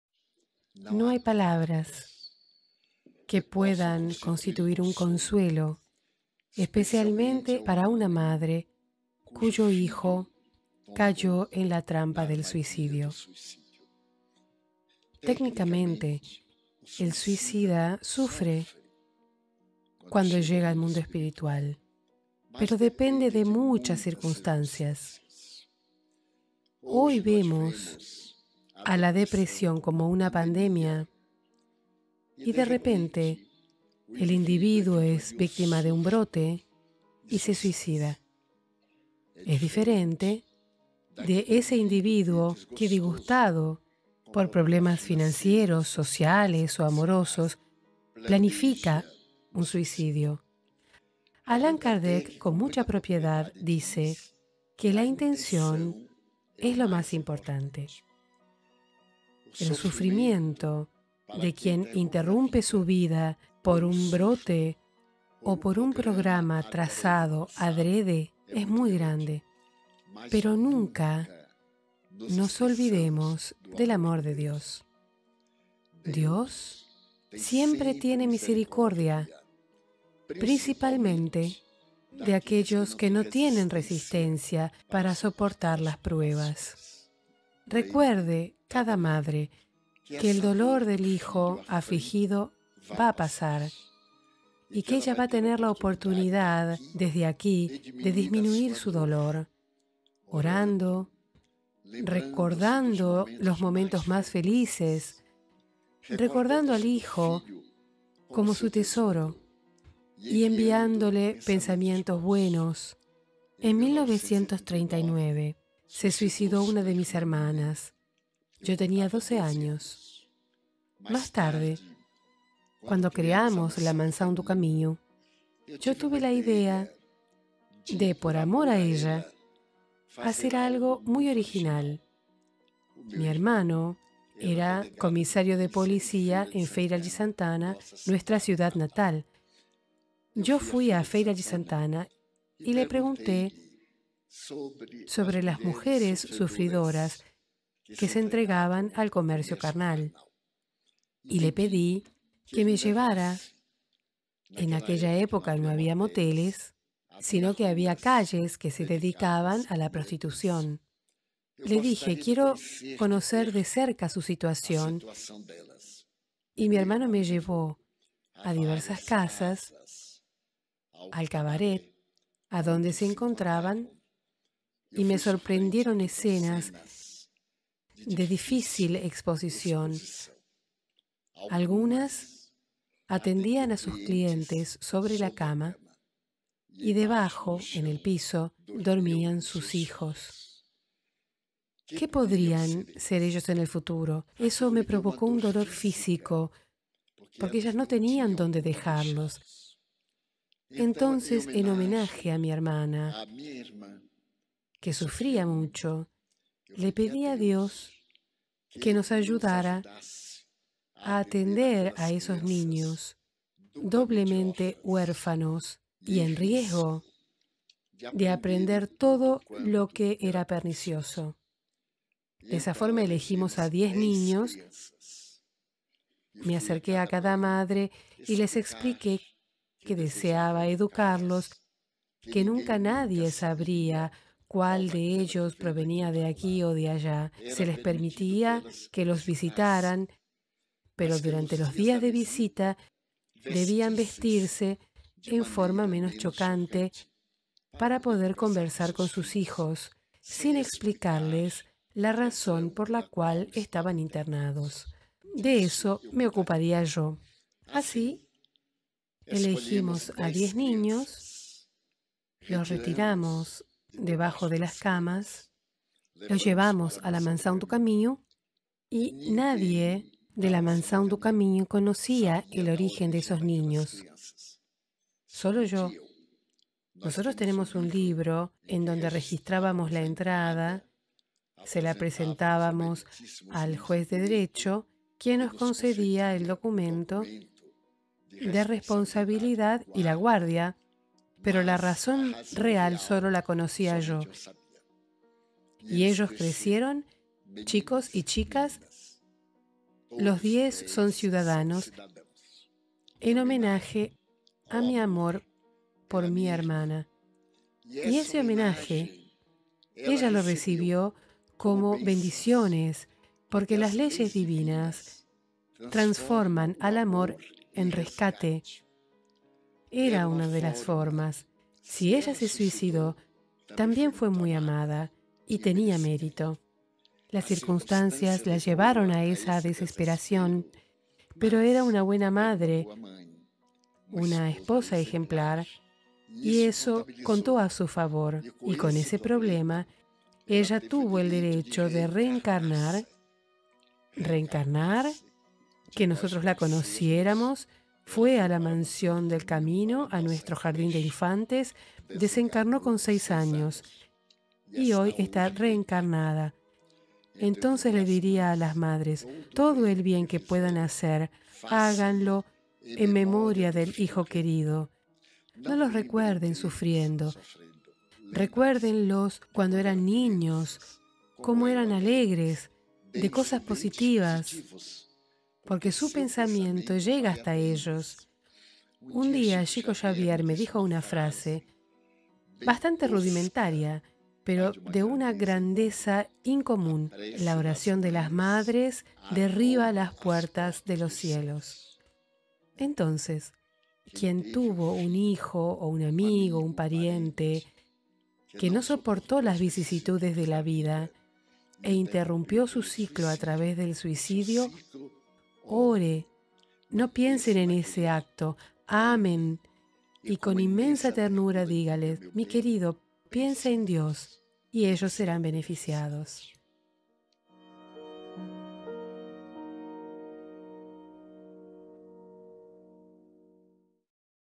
El suicidio en la visión de Divaldo Franco
Emocionante trecho de una de las charlas proferidas por el medium espiritista Divaldo Franco durante la 64ª Semana Espírita de Vitória da Conquista, Bahia, Brasil (09/09/2017). Divaldo nos elucida y consuela citando también el caso de suicidio de la propia hermana, y cómo transformar el dolor en amor.
Para conhecer mais sobre as Obras Sociais Mansão do Caminho que atendem cerca de 5 mil pessoas gratuitamente por dia, acesse

Todos los ingresos de la venta de los libros se destina a las obras sociales de la Mansão do Caminho.Donaciones